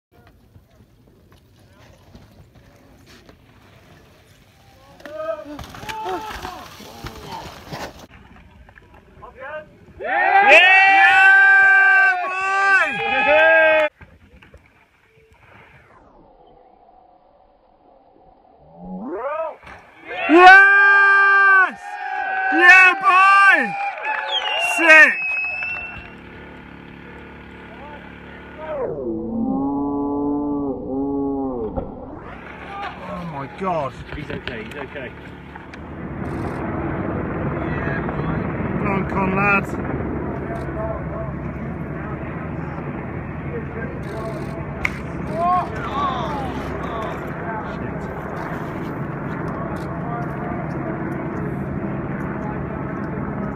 Conor Macfarlane Huge Mountain Bike Crash
This guy is one of the gnarliest dudes I've met, Conor Macfarlane you are one tough cookie, Huge Respect! 👍 Footage made together from @brendog1 and myself.